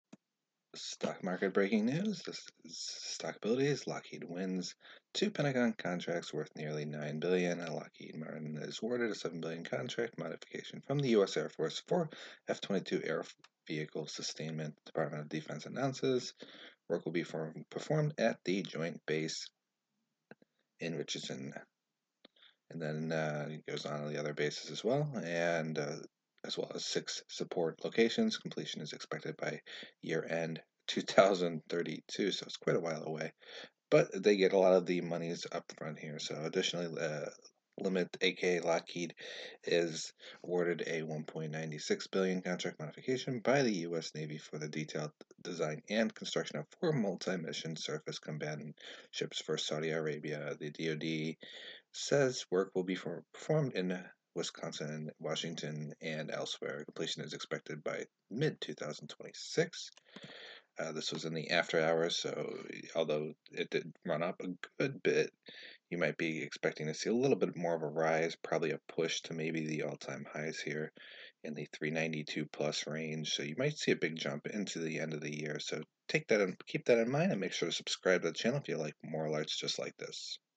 Lockheed wins two Pentagon contracts worth BILLIONS 2020
Lockheed wins two Pentagon contracts worth BILLIONS

Lockheed at it again, locking in even MORE contracts right into end of year.